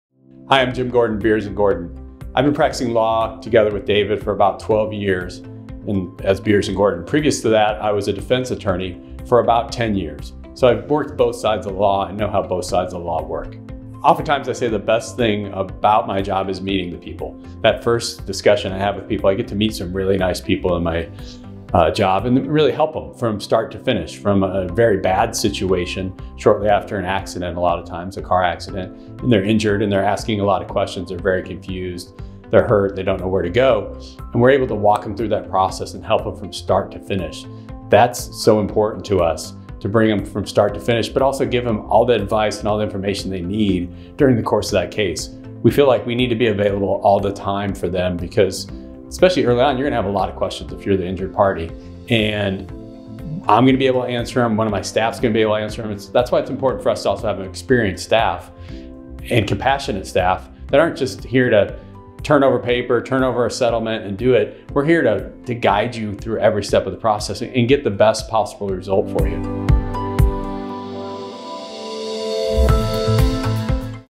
Meet Jim Gordon, Attorney
Jim has unique experience as a personal injury lawyer. Prior to becoming a partner with David, he was a defense attorney for the insurance companies. Jim’s experience working both sides of personal injury cases gives him a perspective that other lawyers lack.

Jim and David realized that the key to helping their clients was to have an experienced staff. They all work together to make sure their clients get the best possible outcome after a traumatic experience.